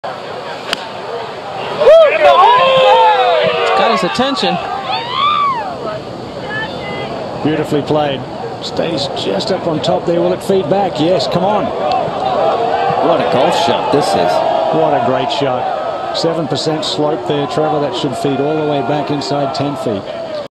Viktor is better from bunkers than you 😱
At the 2023 TOUR Championship, FedExCup Champion Viktor Hovland hit a ridiculous bunker shot from 176 yards away that shows just how on fire he was during the Playoffs.